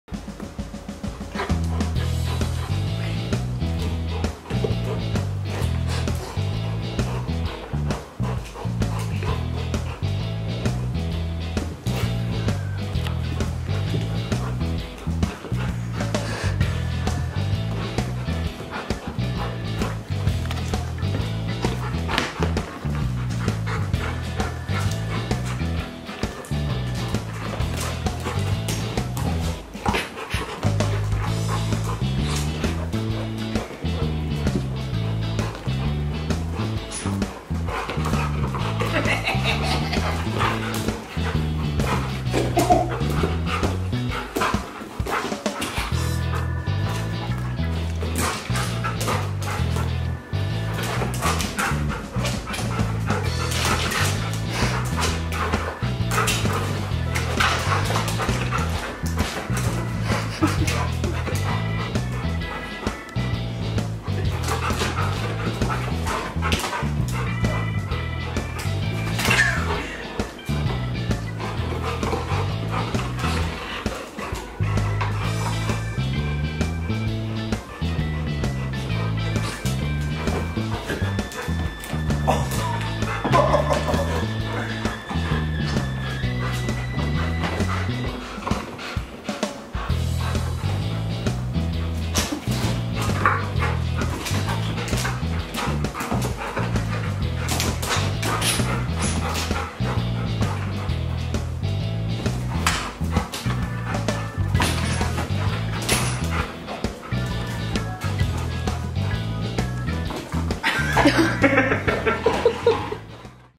How Fun I Play With My Golden Retriever Dog
Friends, look how we are playing with my funny dog Bailey! We hope you enjoyed this video.